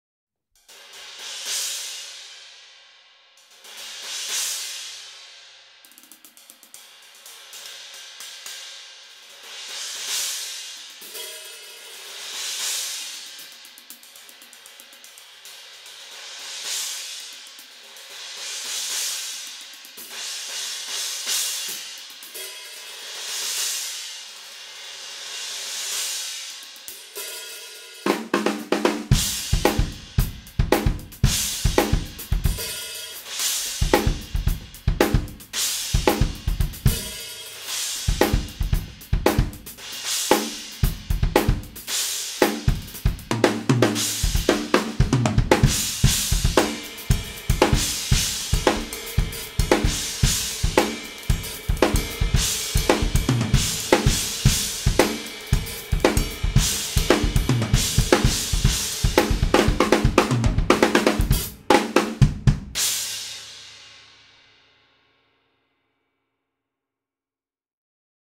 Meinl 16" Byzance Dual Trash Crash Cymbal - 816g (B16DUTRC-1061020B)
Meinl 16" Byzance Dual Trash Crash Cymbal

Played by Marles Flowers Jr.

Other gear used in this video:
• Cymbals (From drummer's L to R): Meinl 16" Byzance Dual Trash Crash Cymbal, Meinl 20" Byzance Vintage Pure Ride Cymbal